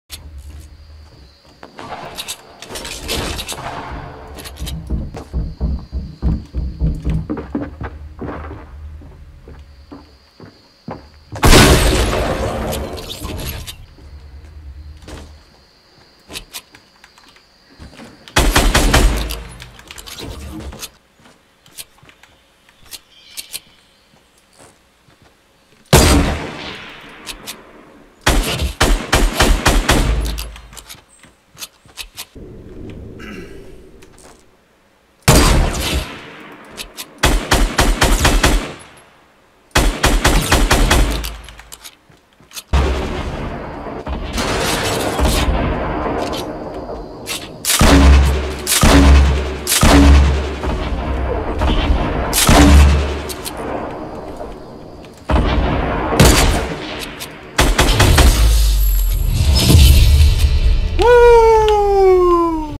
Solo win; 4/16/18
2nd overall win. Kill replay compilation.